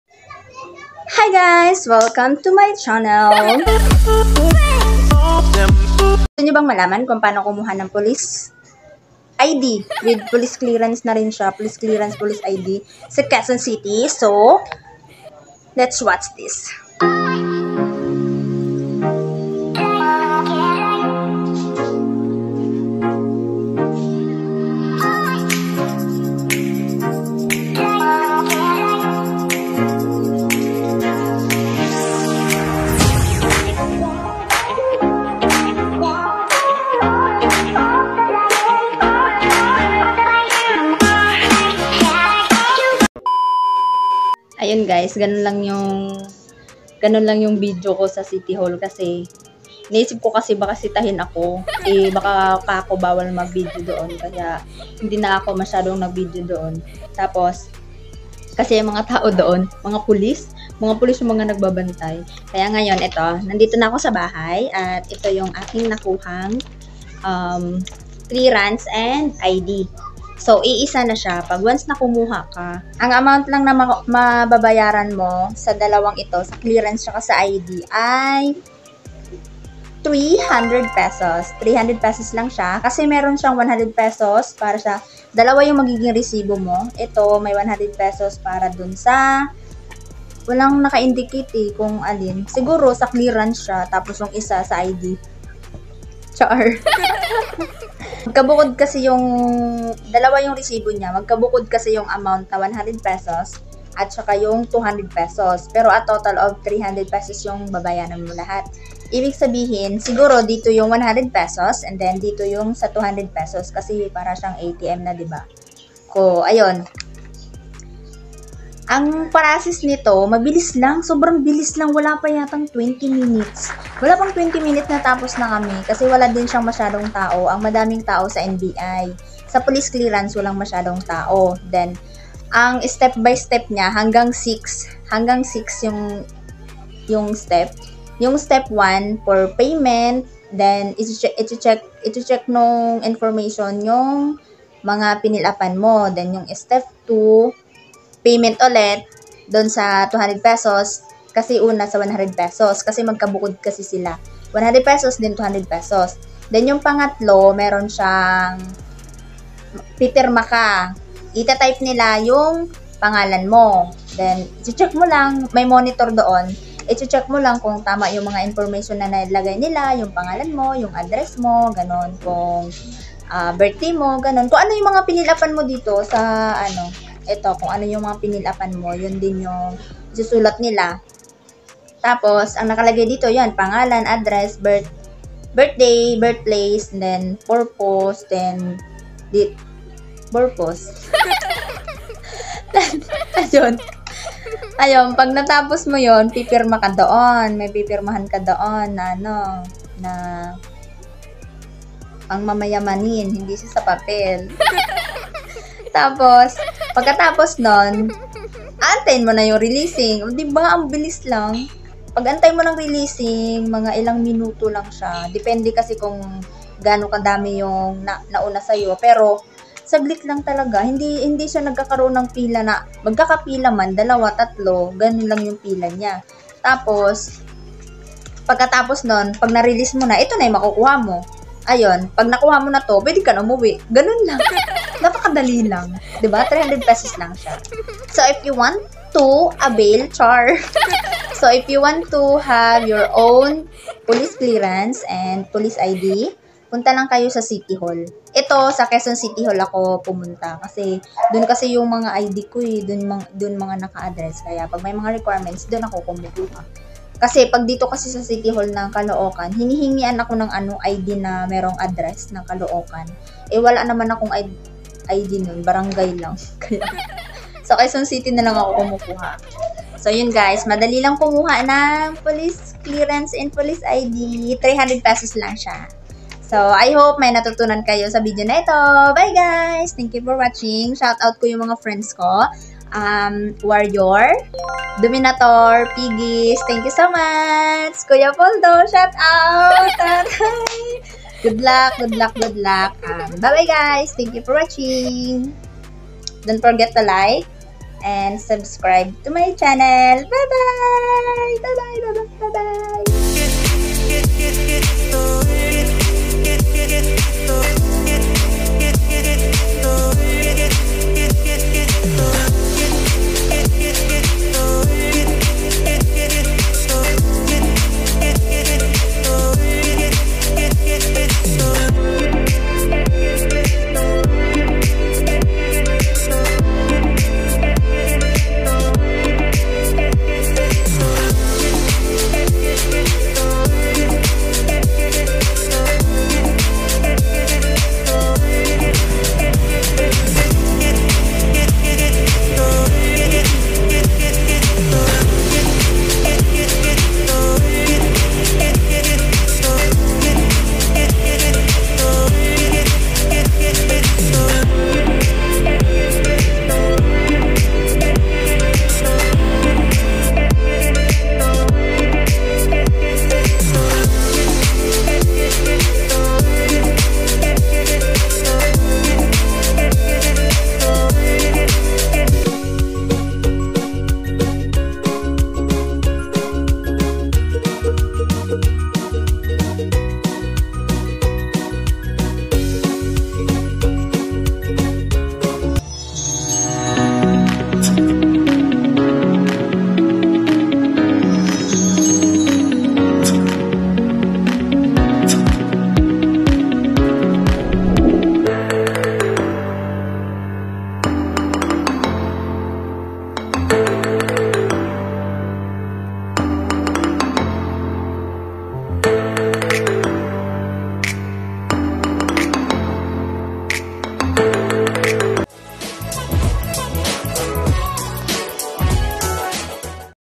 HOW TO GET A POLICE I.D. WITH POLICE CLEARANCE / WHAT IS THE REQUIREMENTS / STEP BY STEP PROCESS
HOW TO GET A POLICE I.D. PLUS POLICE CLEARANCE / STEP BY STEP PROCESS

requirements one valid i.d. with one xerox copy plus three hundred pesos for payment.